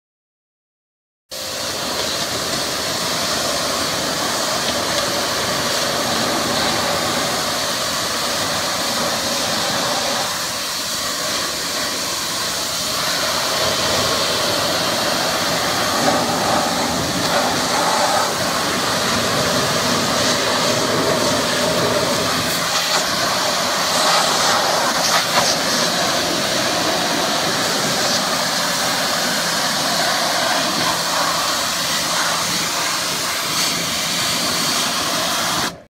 Pressure washing paint off the car
Bought this car back in 2011 in Sandy Eggo (San Diego), it had a shiatty Maaco paintjob but was going to become a racecar anyway.

Sanding this paint off was taking too long so I figured a better way was to just use the pressure washer.